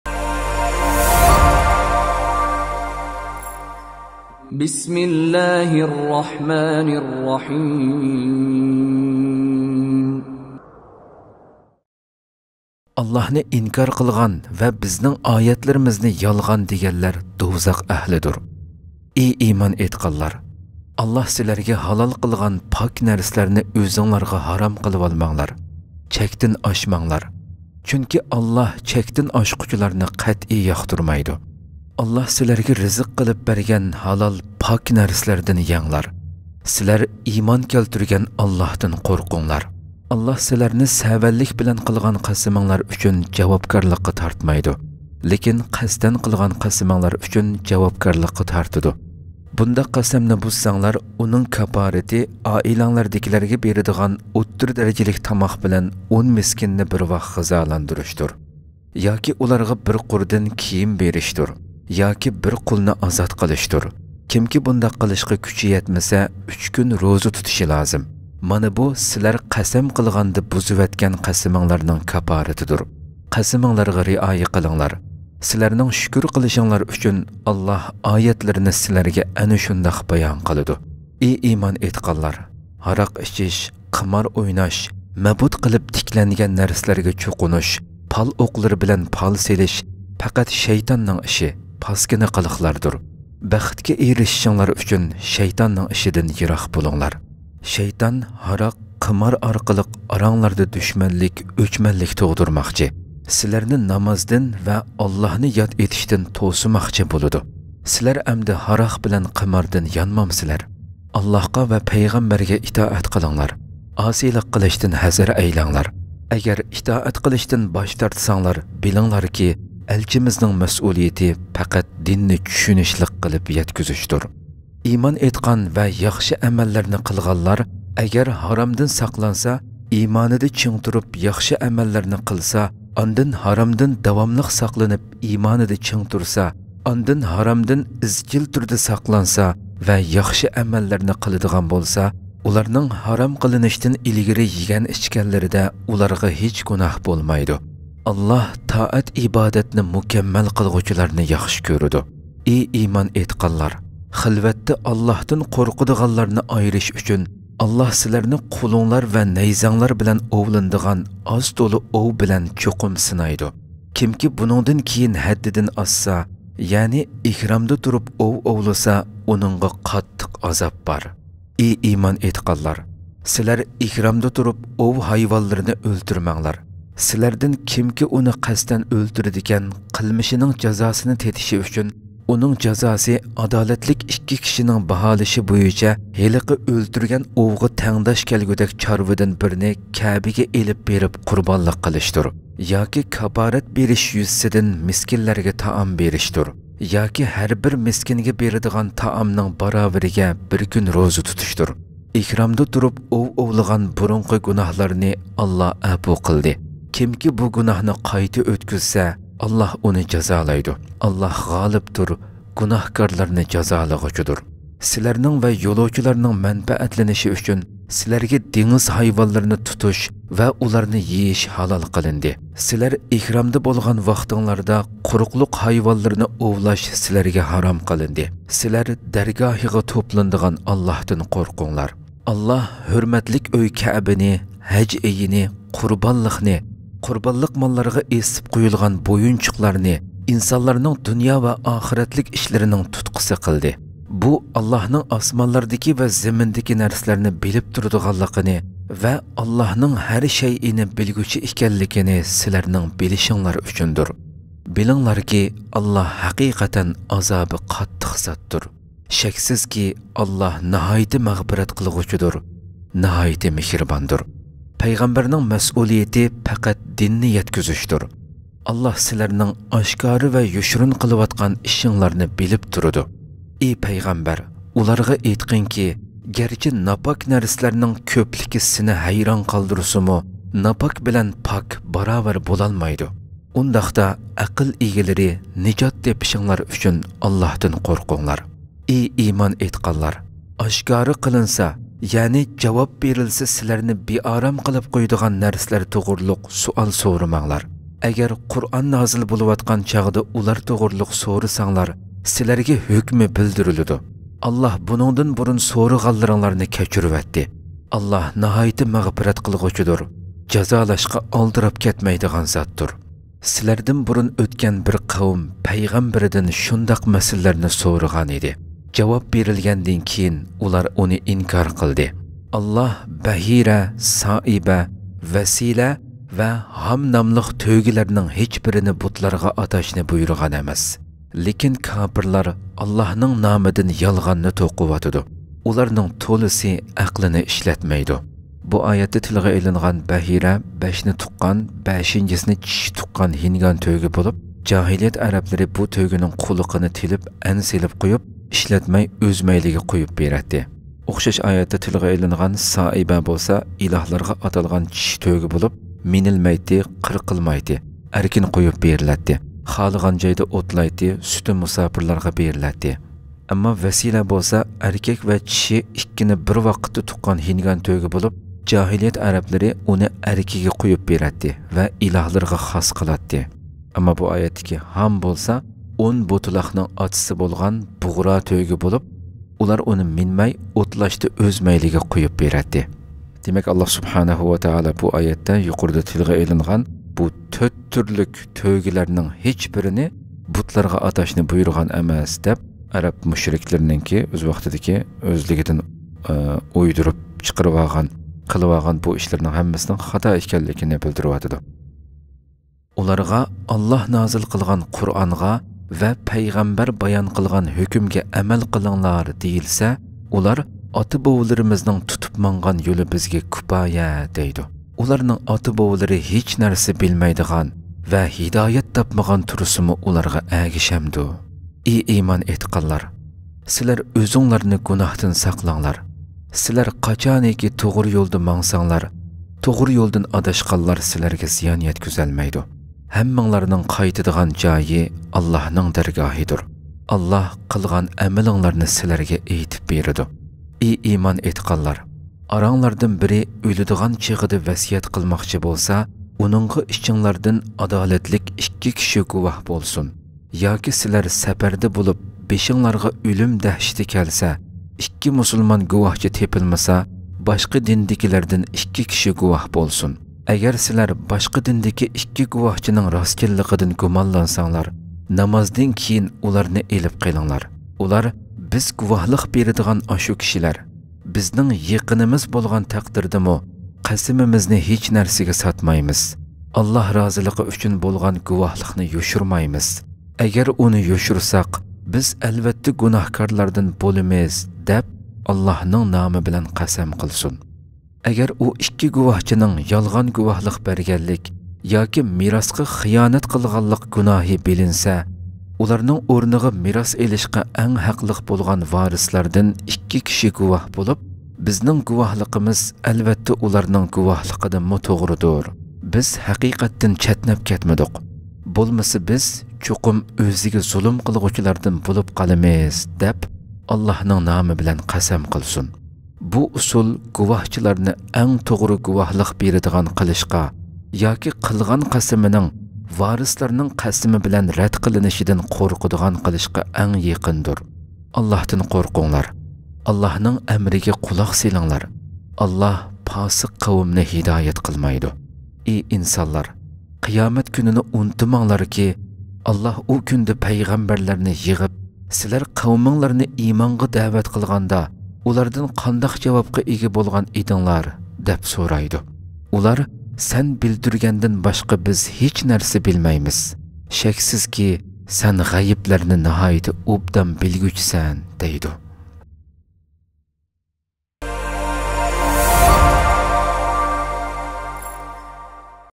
قۇرئان كەرىمنىڭ ئۇيغۇرچە يېڭى تەرجىمىسى | سۈرە مائىدەنىڭ 86-ئايىتىدىن 109-ئايىتىگىچە
Qur'an Kerimning Uyghurche Yengi Terjimisi | Süre Ma'idening 86-Ayitidin 109-Ayitigiche
Қур'ан Кәриmниң Уйғурchә Тәрҗиmиси | Сүрә Mа'идәниң 86-Айитидин 109-Айитигиchә

تەرجىمە: ئۇستاز مۇھەممەد يۈسۈپ قارىھاجىم
ئاۋاز: ئابدۇللاھ يالقۇن